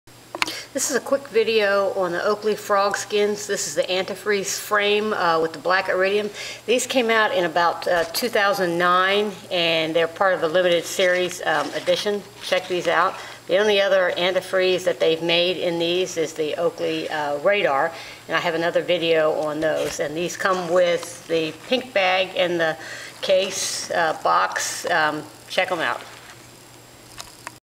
Oakley Anti-freeze Frogskins, Limited Edition, Brilliant Green
Another of the limited edition Frogskins by Oakley.  These came out in 2009 & have long been sold out.  Black iridium lens, frame is the awesome Anti-freeze.